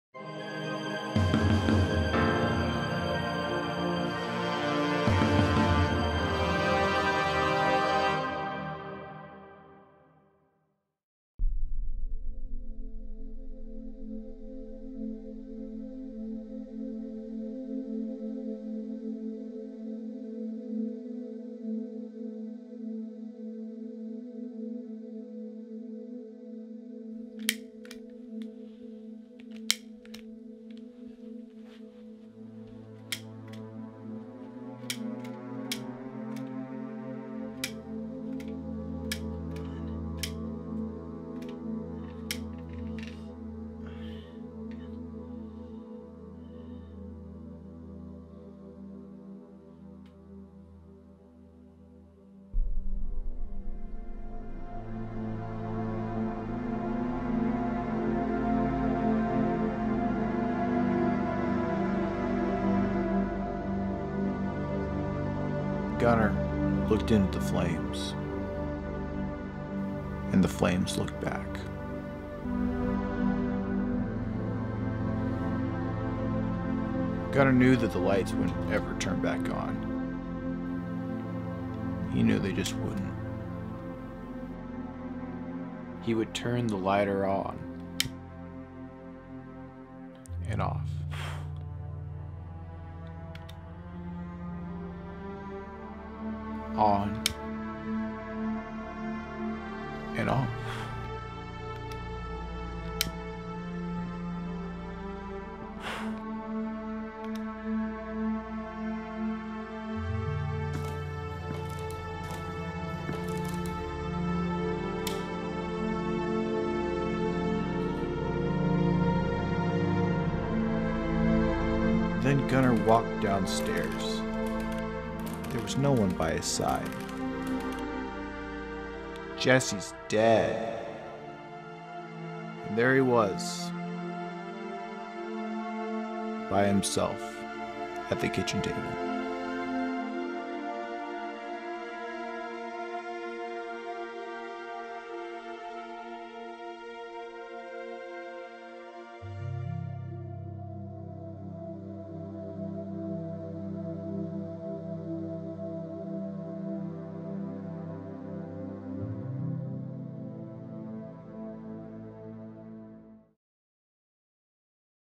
Candle II
It's Gunnar's candle, Gunnar's light in this darker and moodier narrative.

Cast:
Gunnar Womack
Isaac Kjose